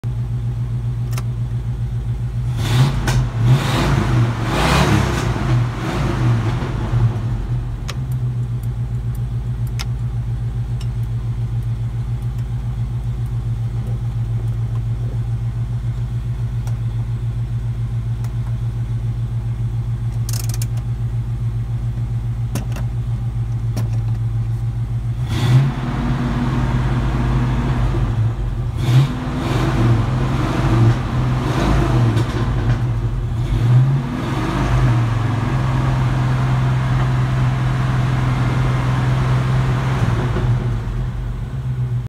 Final in-car test of overhauled gauge cluster and LEDs
Testing the updated gauge cluster with LEDs after my second install following a couple of hiccups that needed troubleshooting and fixing.